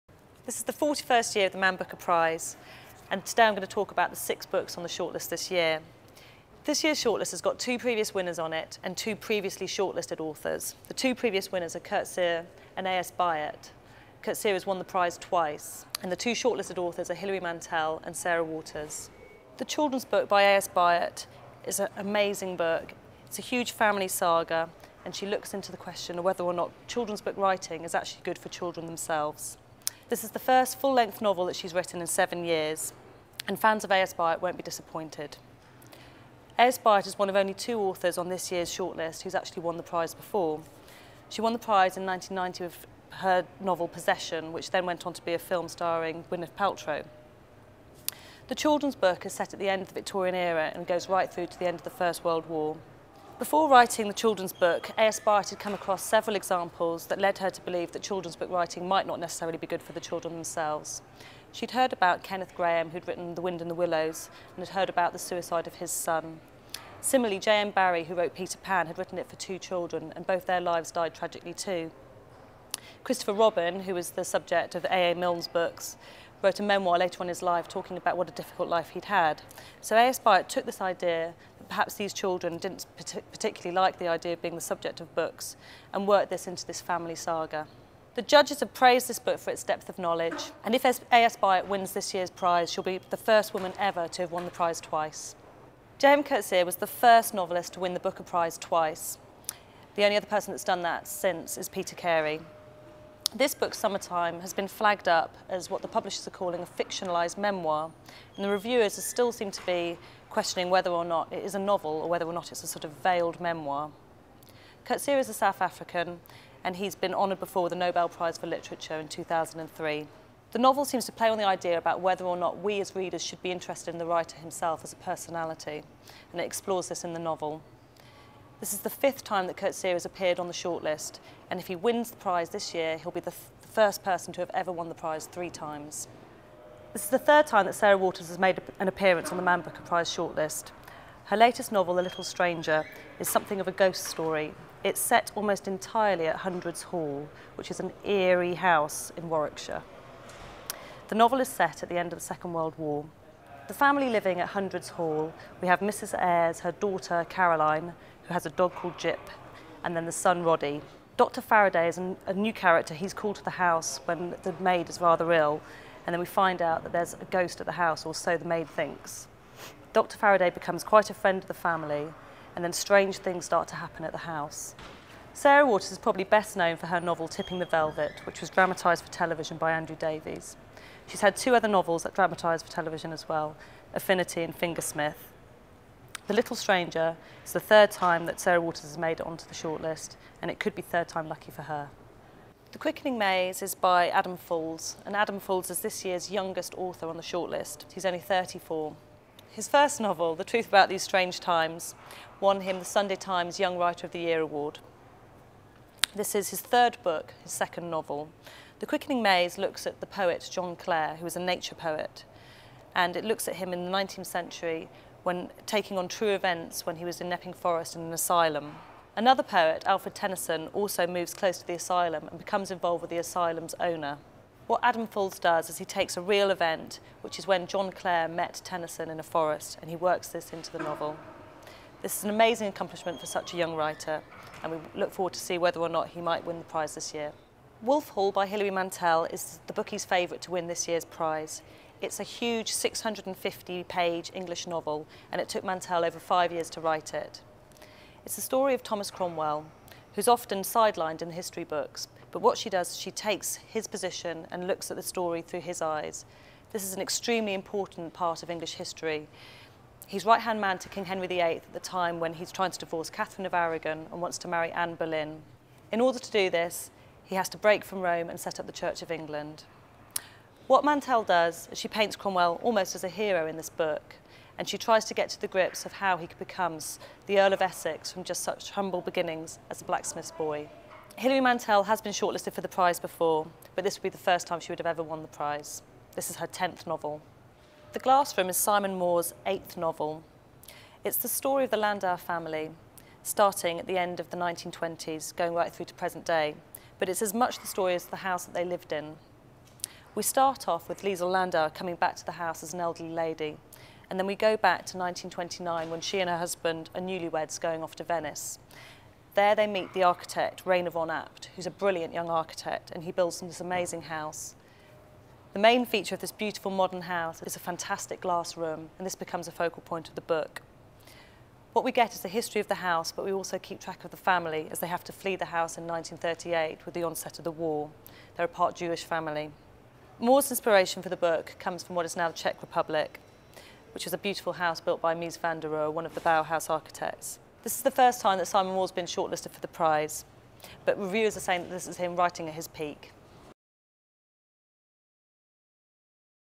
Summary of the Man Booker Prize nominations
Man Booker Prize Content Editor, Sophie Rochester talks through this year's nominations ahead of next week's Award Ceremony.